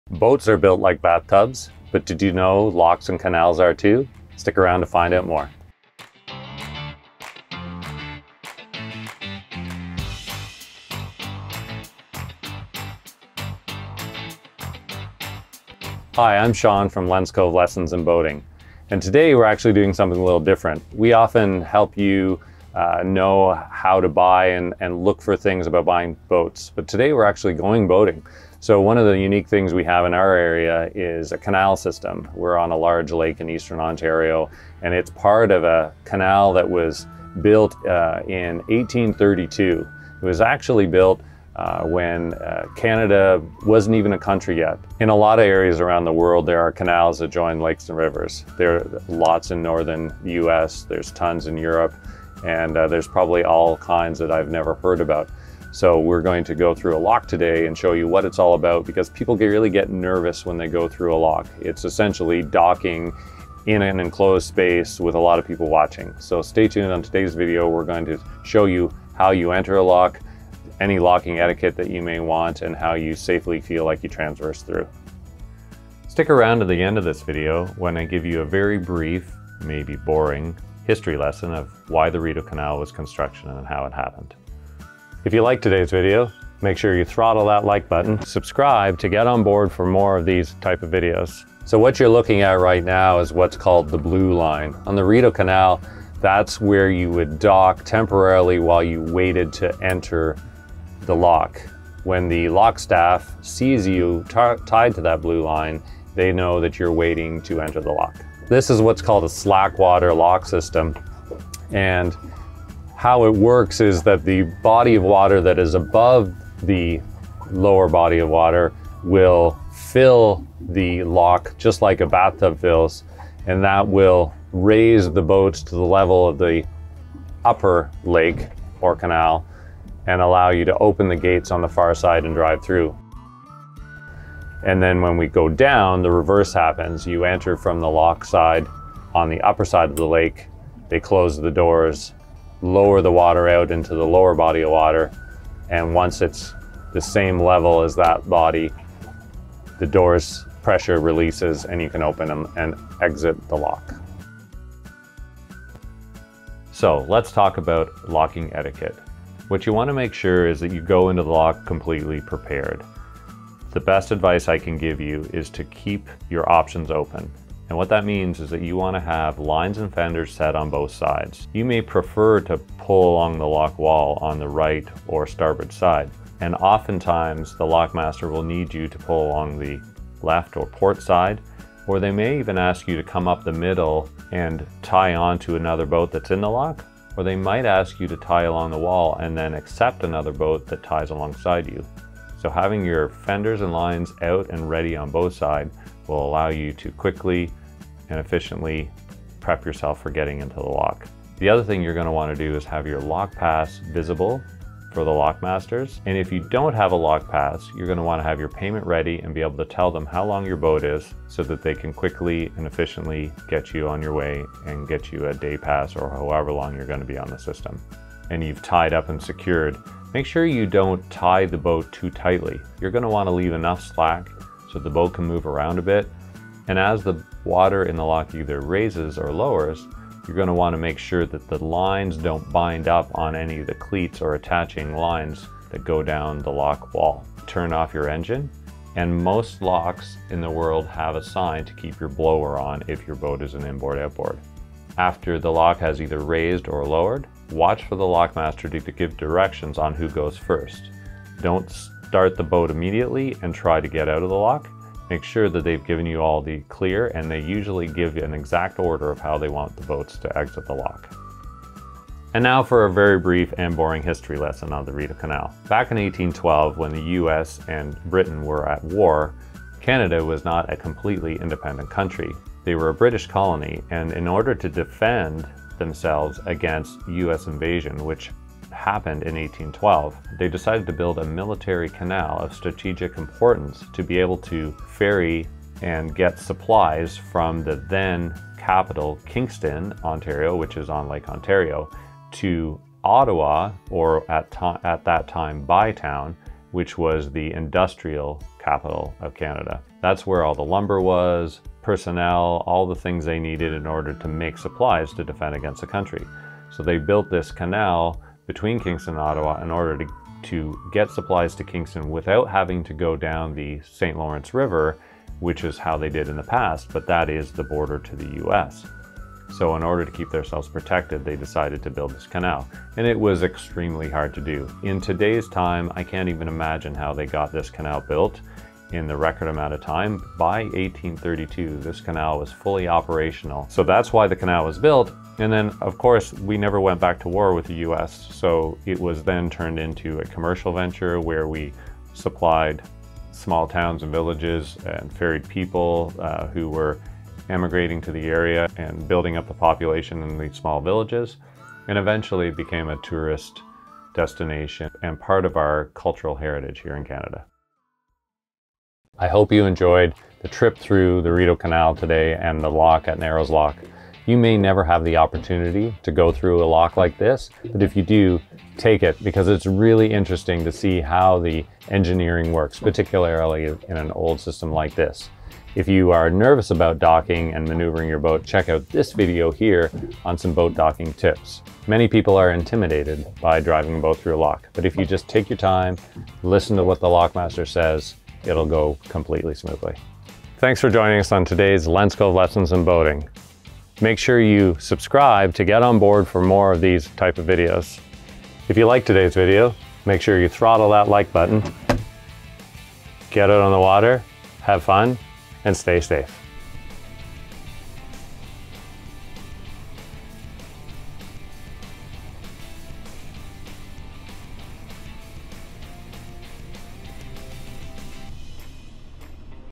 Boating Through a Lock
Have you ever wondered what boating through a lock is like? Some people are very nervous or intimidated by the idea and because of that leave all kinds of great boating areas undiscovered. We have a gorgeous lake and canal system here on the Rideau Canal in Eastern Ontario. So get out and explore it! In this video we will cover:

Intro 0:00
How to enter the lock 1:30
Locking etiquette 2:36
Brief Rideau Canal History 4:21